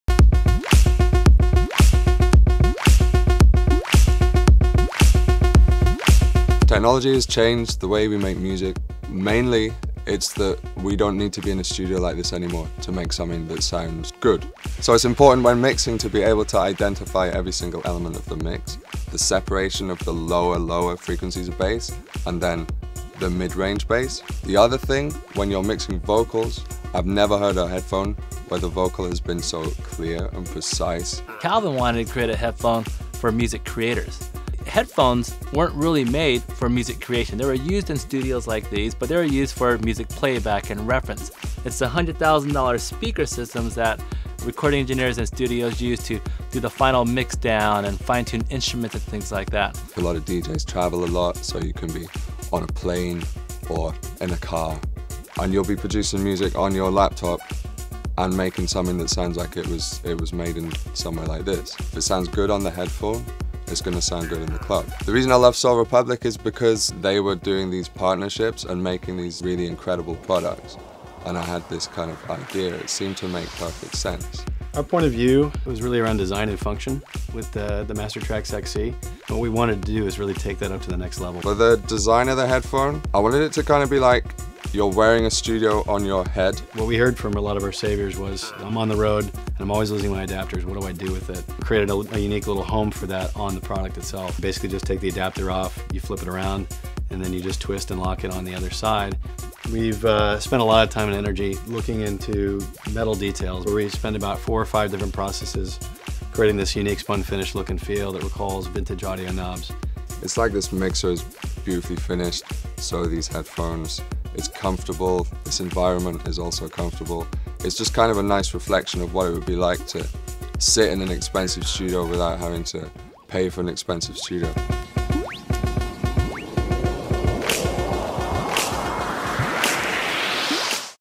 SOL REPUBLIC Master Tracks XC, Studio Tuned by Calvin Harris
We partnered with Calvin Harris to create Master Tracks XC, headphones for both music lovers and creators. Check out Calvin Harris and SOL REPUBLIC CEO Kevin Lee discuss these amazing new headphones.